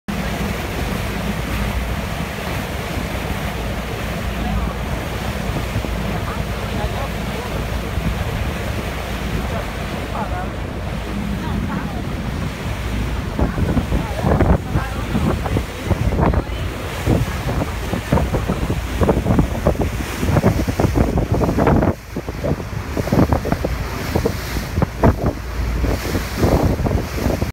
Italy - Liguria - The five lands by boat
Juillet 2016 - Visite des cinq terres en bateau, Ligurie, Italie